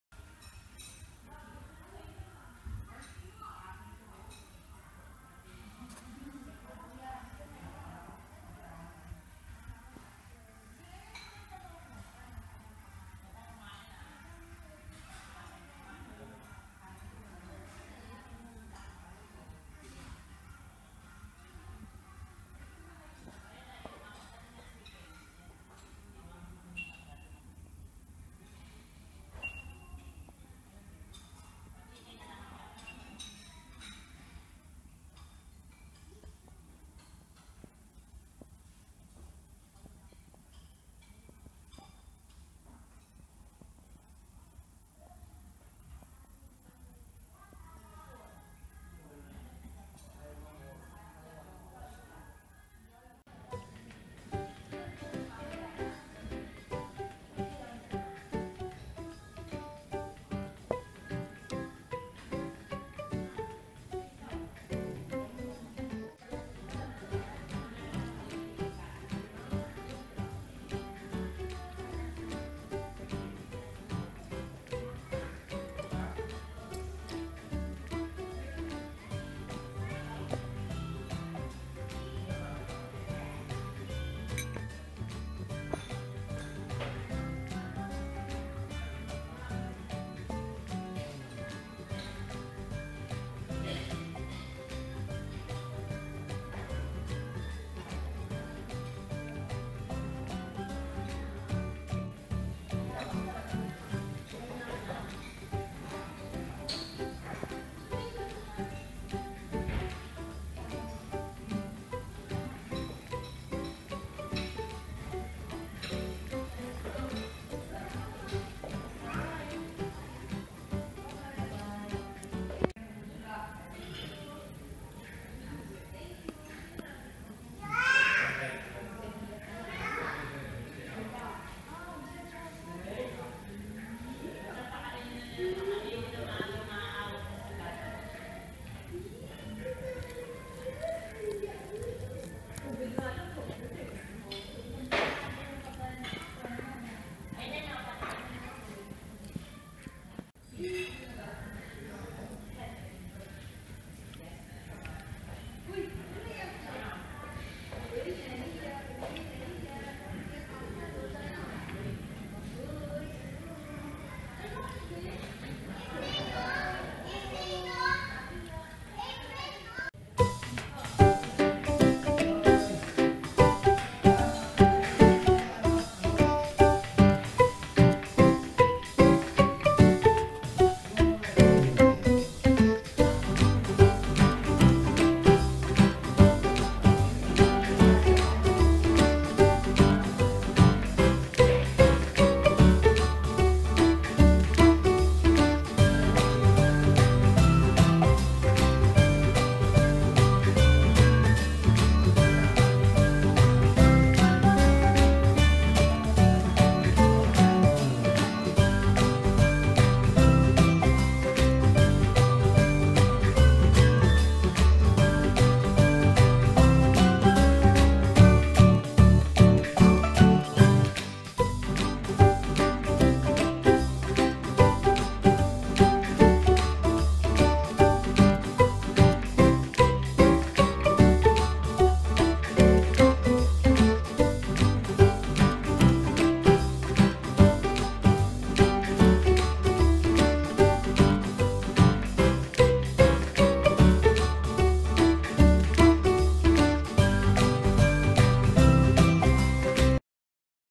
The Best Grilled Chicken At Bonchon Cambodia
It's very delicious BBQ chicken ever. It impresses me a lot about its taste as I put it inside my mouth. I can't describe my mood to make you guys believe what I've expressed. Therefore, I recommend you guys should try it someday in Phnom Penh the capital city of Cambodia.